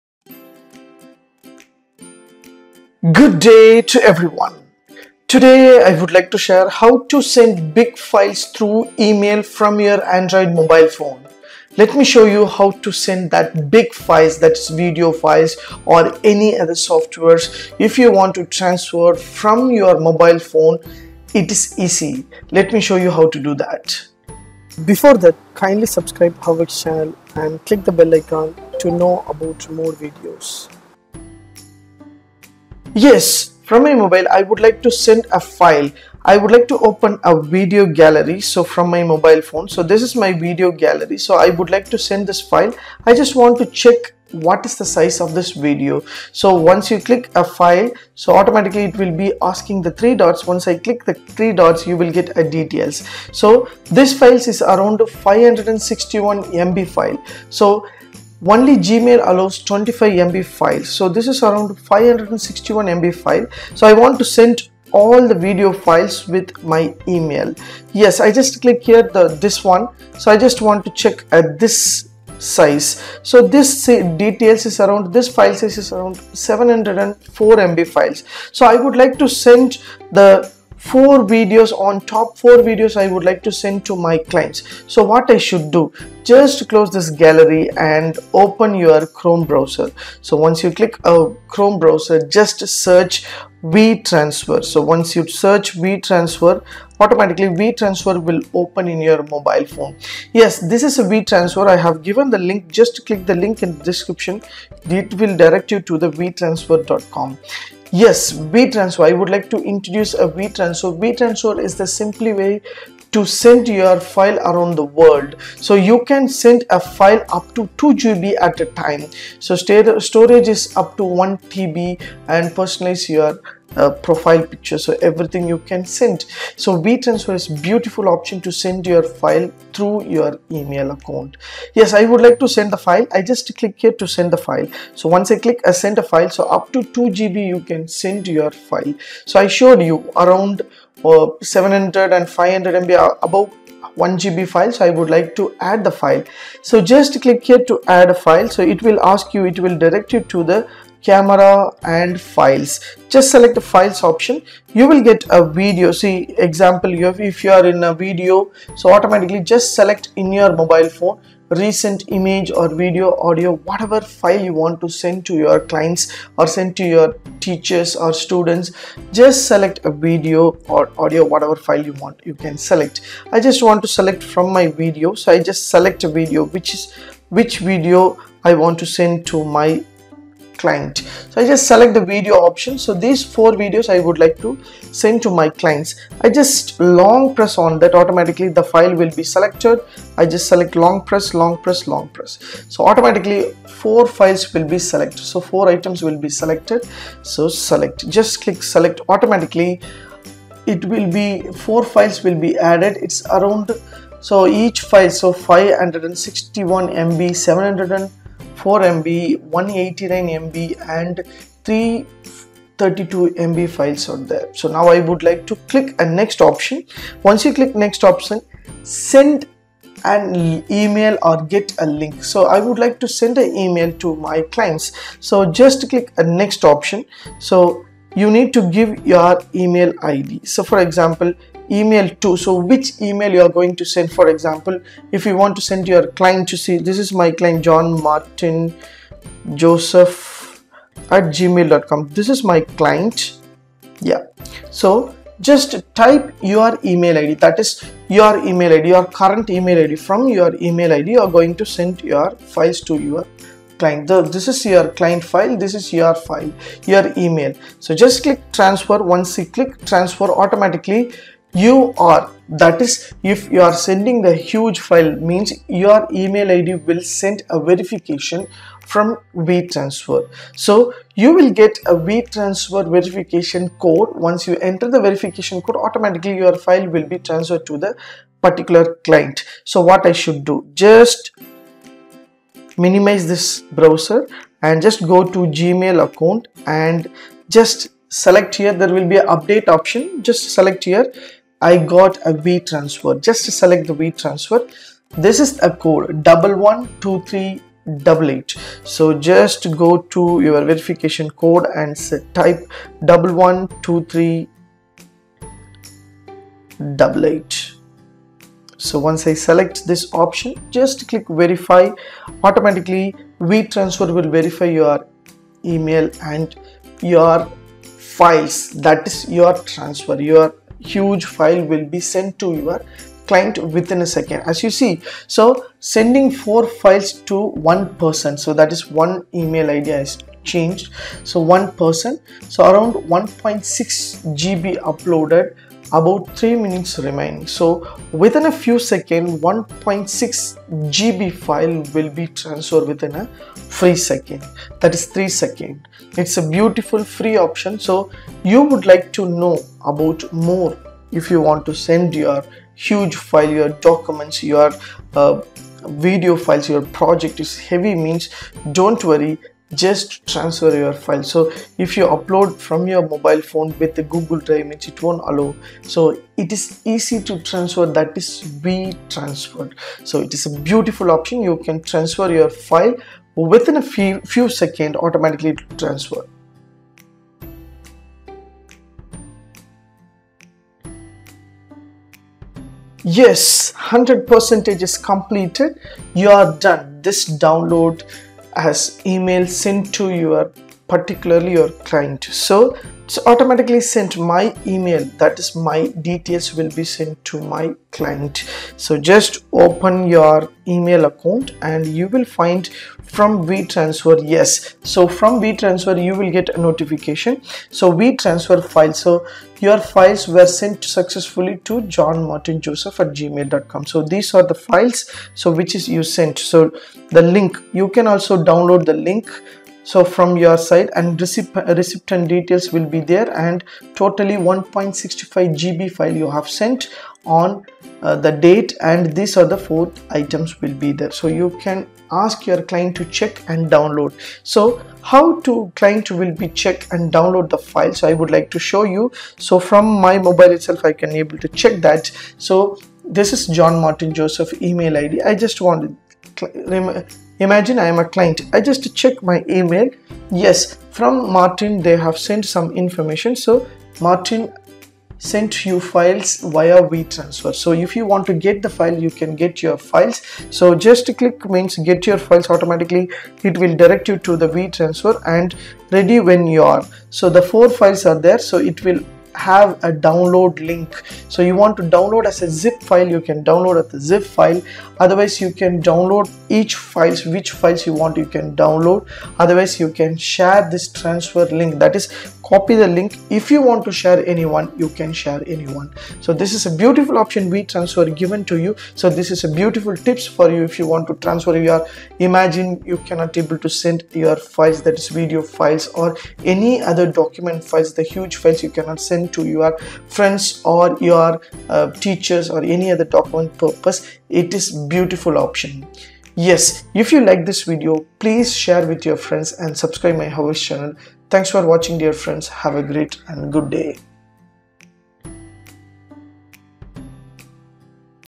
How to send big files through email android
This video tutorial will help you How to send big files through email android
simplest way to send your files around the world. Share large files up to 2GB for free.

00:00 Introduction
00:40 How to Send Big Files from mobile using Gmail
06:39 How to Download big files from mobile using Gmail